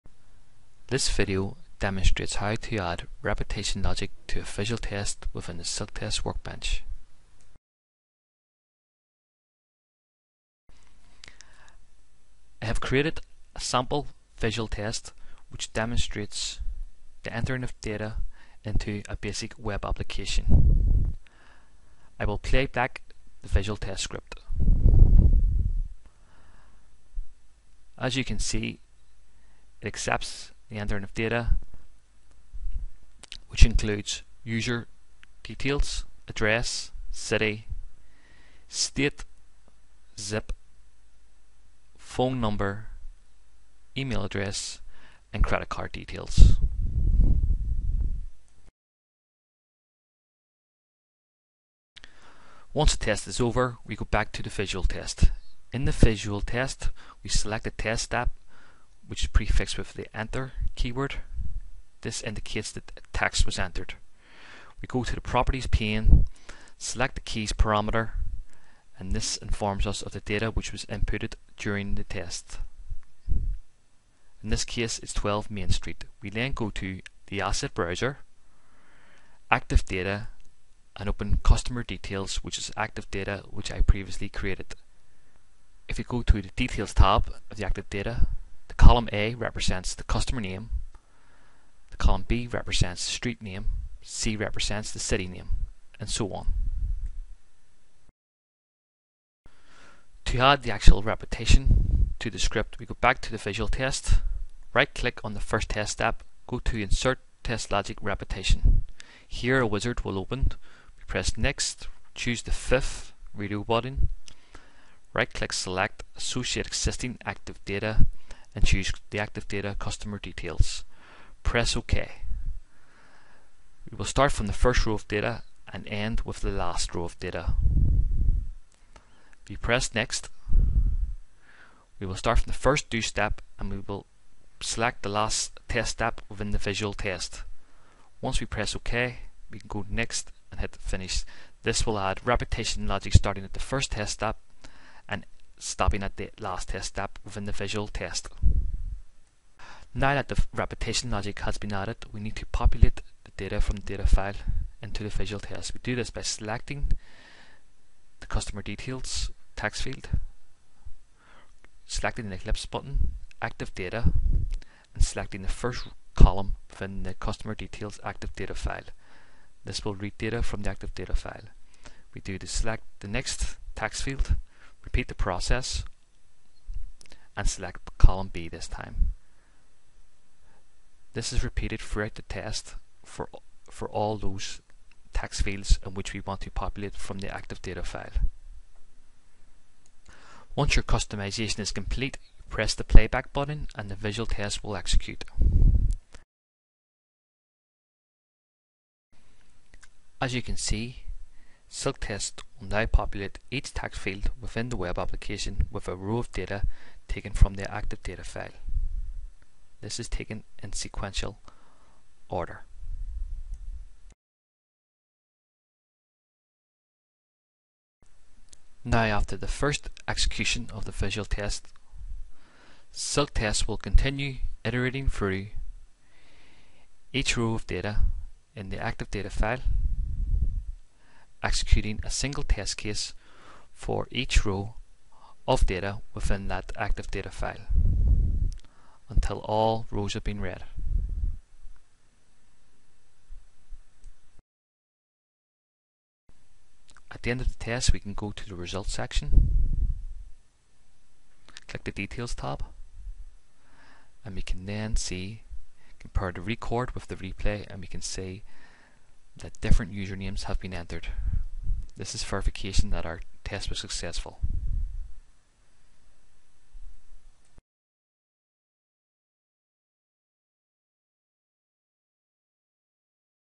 Inserting Repetition in a Visual Test in SilkTest Workbench
This video demonstrates how to insert repetition in a Visual Test in SilkTest Workbench.